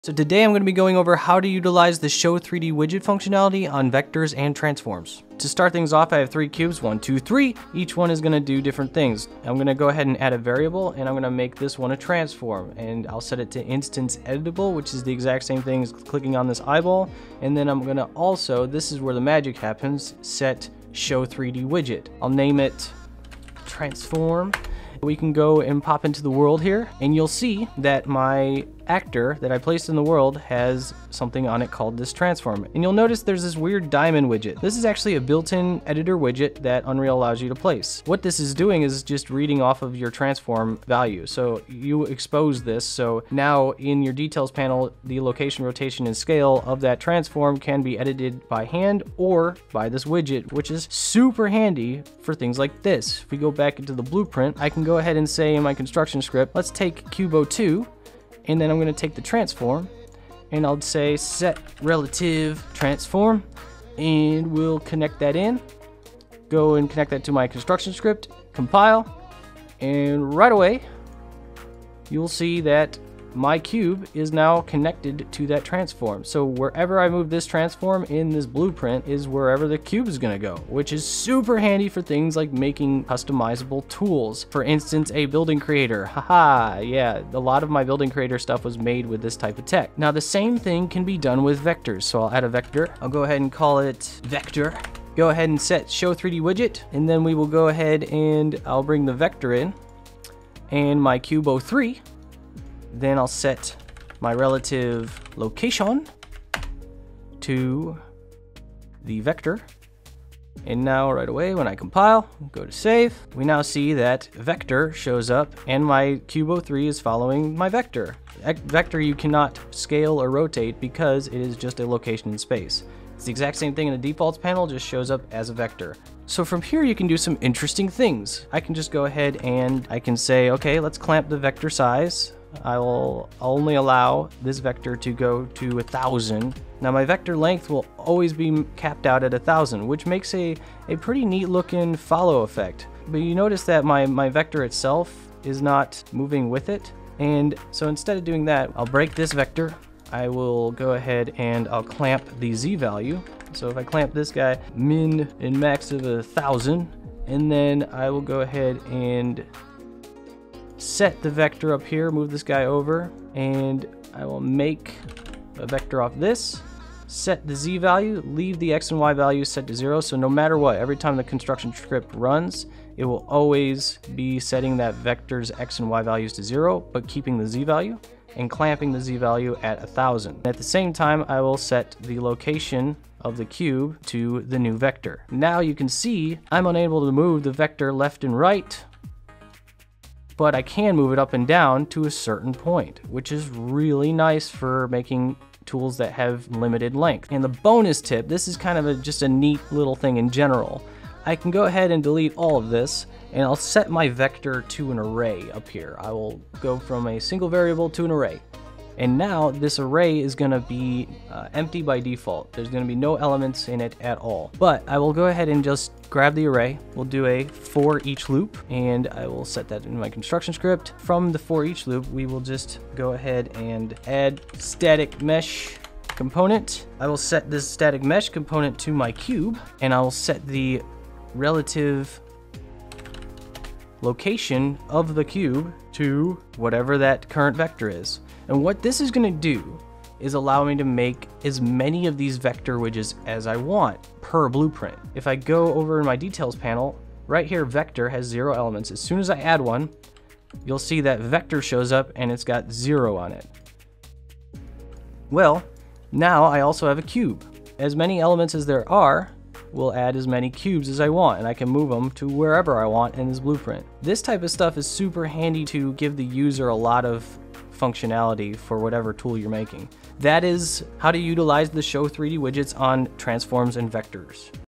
UE4 How To Use Blueprint 3D Widgets on Transform Properties.
Use Show 3D Widget is a very powerful tool for anybody making tools in blueprint. Today's video goes over a couple of neat ways to use it as well as the basics of how to get started using it.